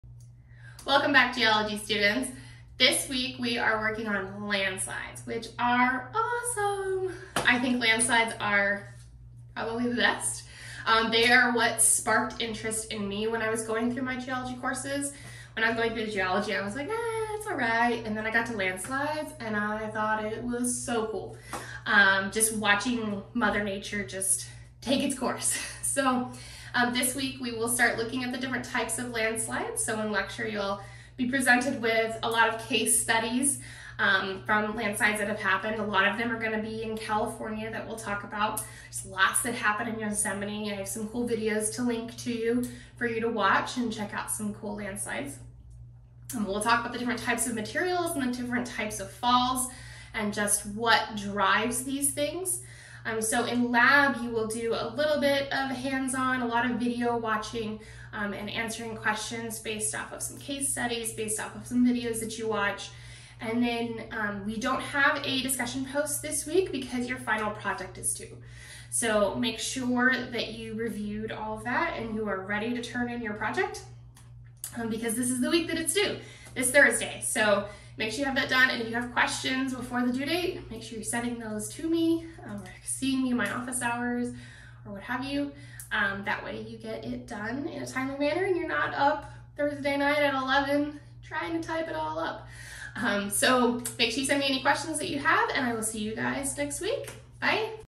Earth's Geosphere: Mass Wasting
This is an introduction to your Mass Wasting Module.